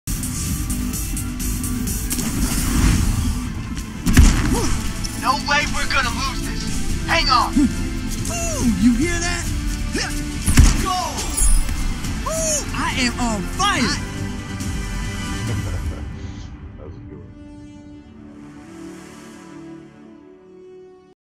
Josh Terror as Lúcio
That's me with the game saving goal with 2.6 left on the clock!!  We went on to win in overtime.